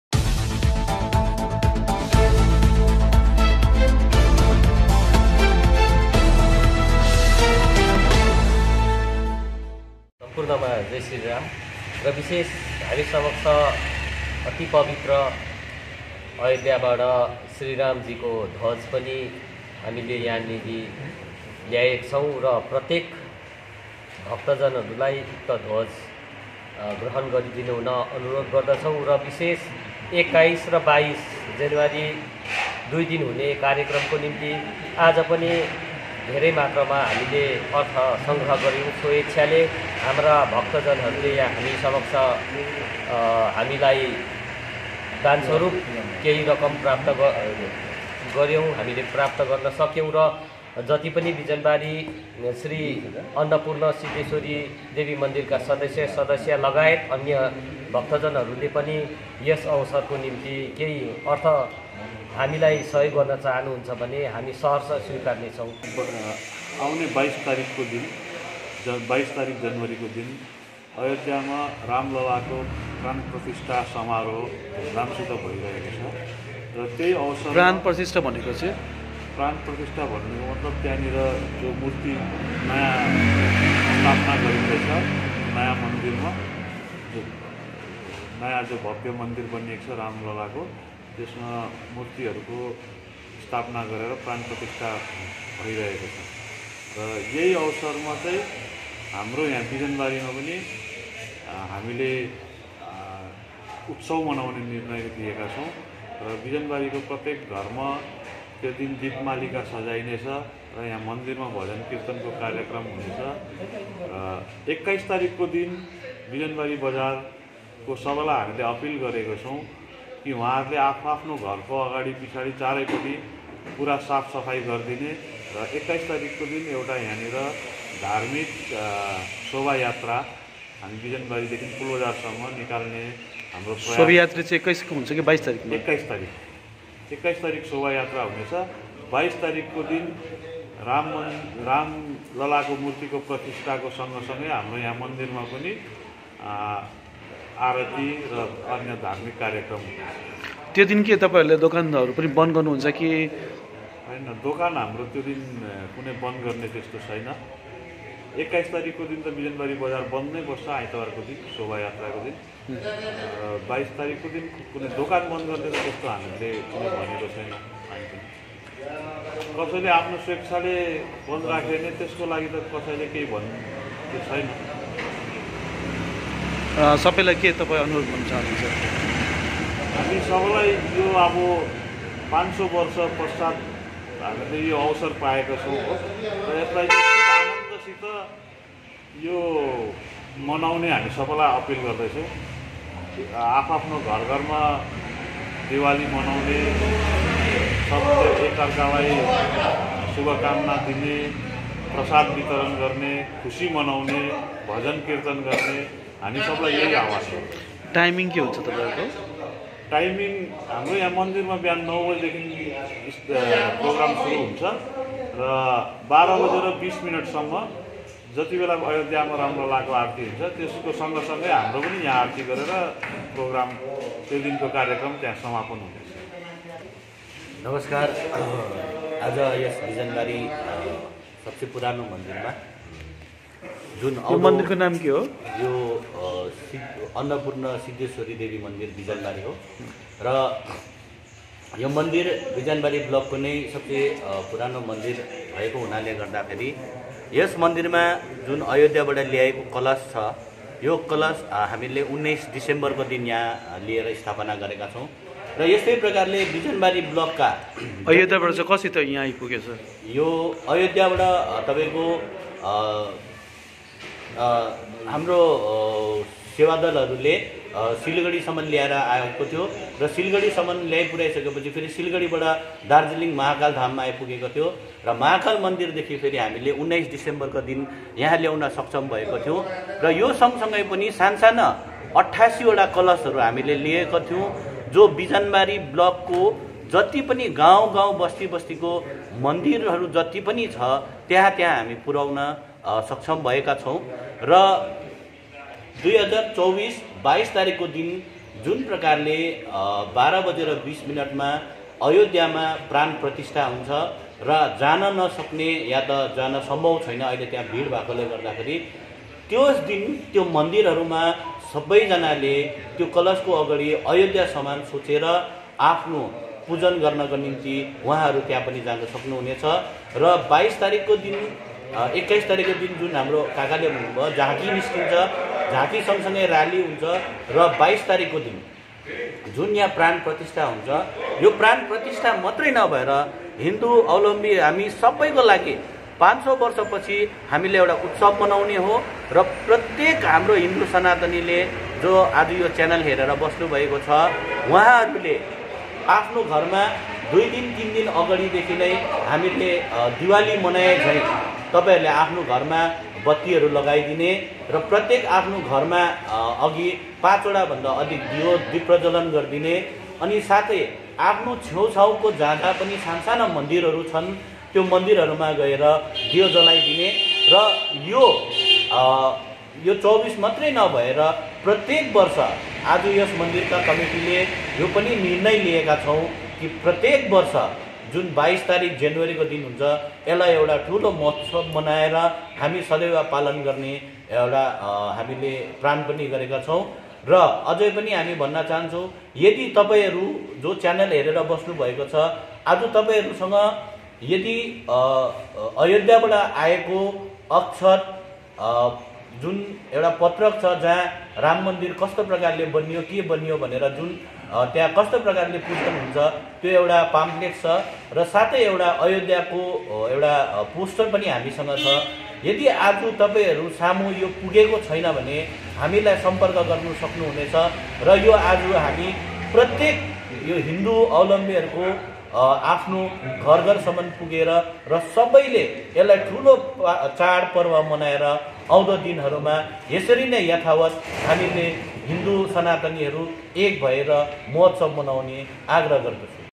DARJEELING NEWS || अन्नपूर्णा सिद्धेश्वरी देवी मन्दिर बिजनबारीमा भगवान राम उत्सव मनाउने!!!
बिजनबारी : आगामी 22 जनवरी 2024 को अयोध्यामा भगवान रामको मूर्ति स्थापना गरिने भएको छ। यसै अवसरमा अन्नपूर्णा सिद्धेश्वरी देवी मन्दिर बिजनबारीको आयोजनामा आगामी 21 अनि 22 जनवरी 2024 को दिन भव्यता साथमा दुई दिने उत्सवको रूपमा मनाउने निर्णय गरेको छ। स्थानीय अन्नपूर्णा सिद्धेश्वरी देवी मन्दिरमा पहिलो दिन भगवान रामको पूजासँगै शोभा यात्रा गरिनेछ भने दोस्रो दिन भगवान रामको आरती अनि भजन-किर्तन गर्नेछ भनी आयोजक समितिले जानकारी गराएका छन।